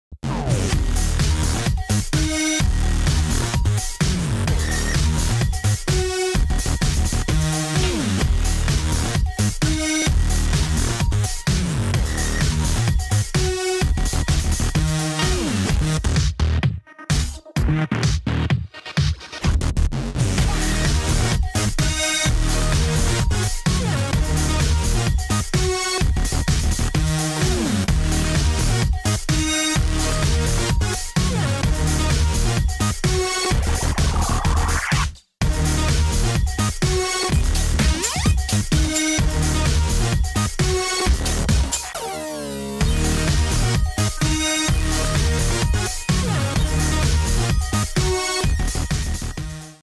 BSP Castanet time lapse
Castanets are in production regularly here at BSP Headquarters. Instead of talking you through the whole boring process, we thought we'd show everything from start to finish in less than a minute...ready, set, go!